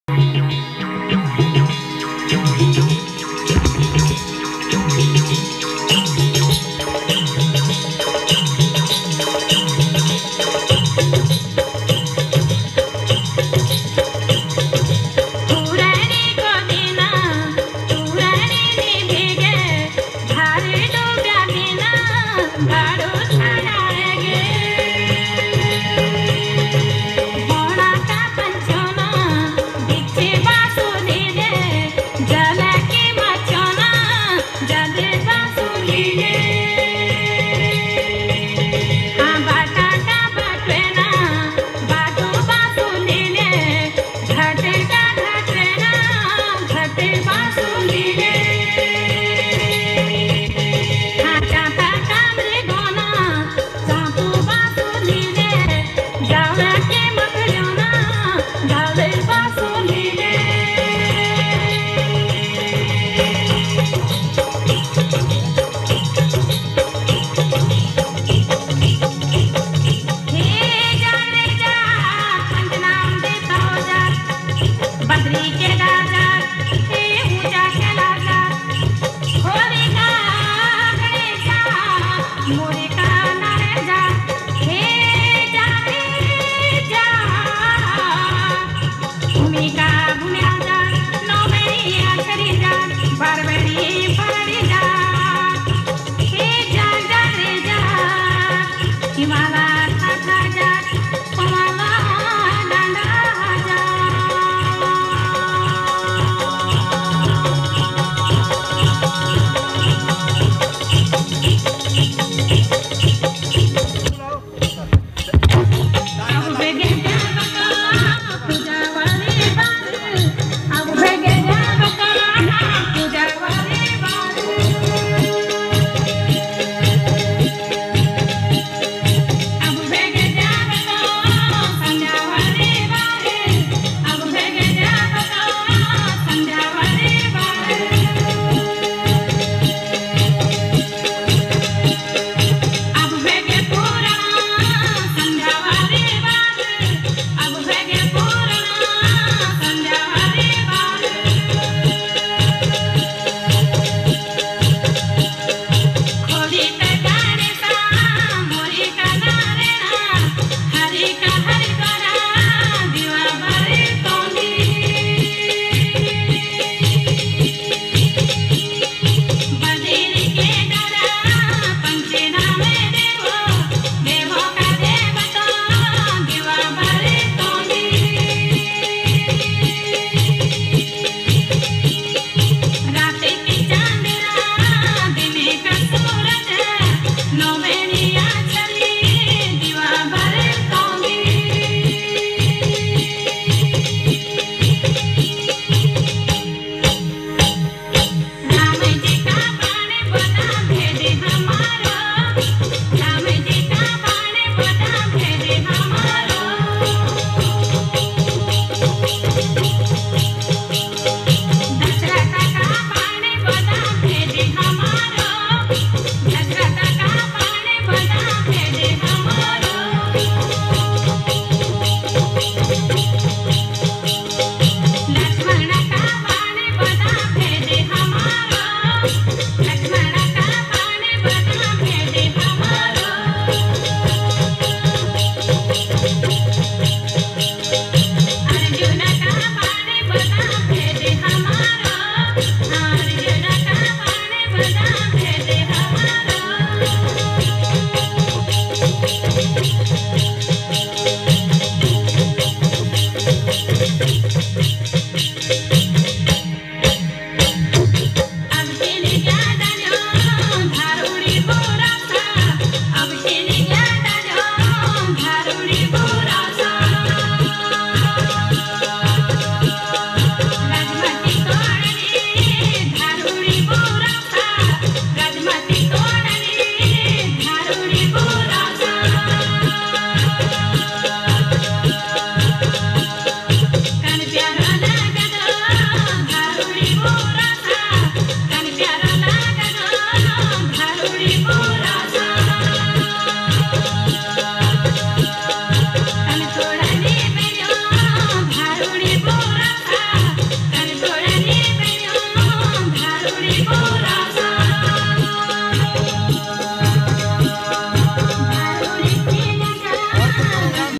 gadwali dance by school girls
beautifull gadwali cultural dance performance in intermediate school uttrakahand
Orignal song by - pummy nawal ji